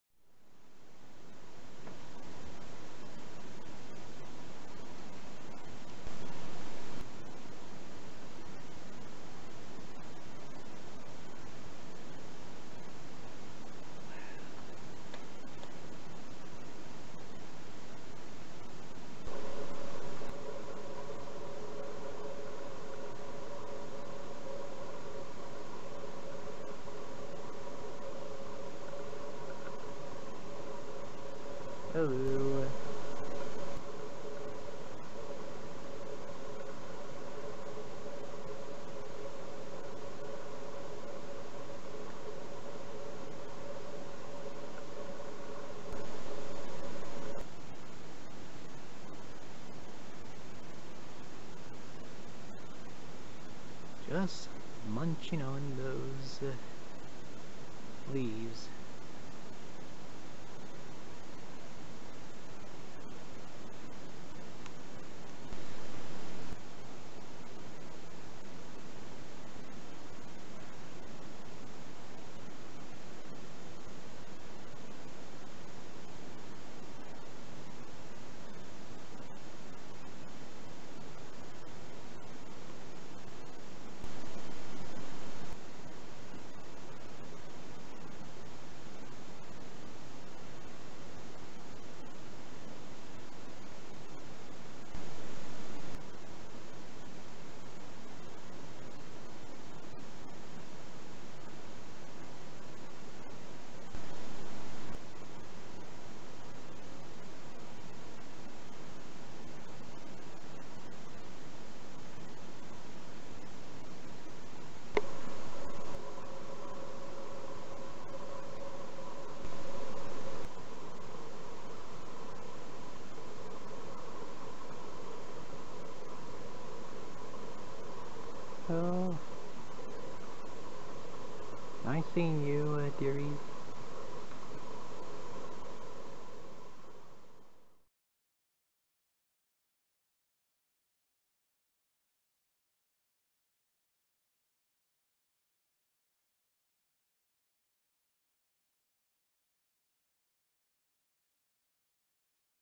Deer in the Snow and Woods
Last week, I saw two deer appearing in the back yard in the snow. I bet this would a holiday sight moment of wildlife watch, due to them both having antlers. Who knows, they maybe brothers.